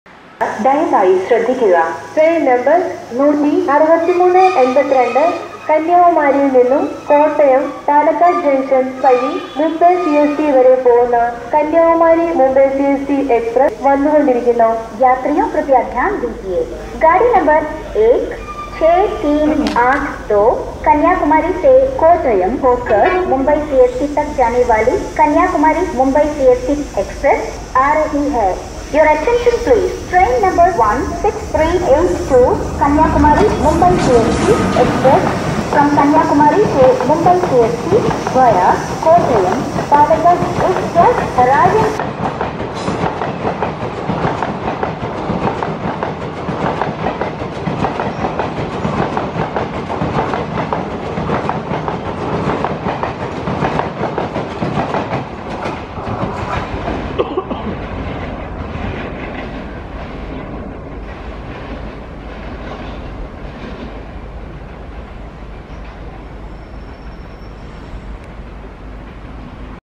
Coming Back Soon 😍 LHB Kanyakumari - Pune Jayanti Janata Express
16382 Kanyakumari - Mumbai CSMT Jayanti Janata Express accelerates towards Ernakulam Town after clearing the Permanent Speed Restriction (PSR) over the bridge near Tripunithura railway station, Kerala.

Erode (ED) WAP4 22570

* Jayanti Janata Express haven't started service since pandemic & expected to start service by April 2022 depending on the availability of LHB coach. While restarting service, it will run only till Pune instead of Mumbai. LHB coaches (22) will replace the existing ICF coaches (17).

**this video captured in 2018.